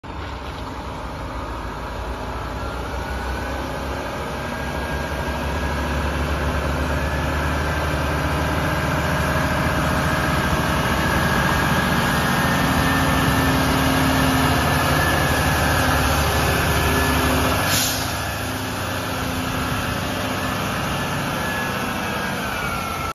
5,000 gallon water truck by HEC. New heavy spec Mack Granite chassis. Front spray bar w/ flushers
New heavy spec Mack chassis for off-road use, mining/construction application
New HEC 5,000 gallon heavy duty mining version, V bottom water tank
6 spray heads
Remote Control water cannon
Hannah rear hose reel
Epoxy coated inside